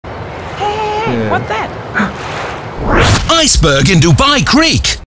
RTA Waterbus Teaser
Iceberg on Dubai Creek! Teaser TVC to introduce the new cool waterbus for RTA Dubai produced by CLIPPS.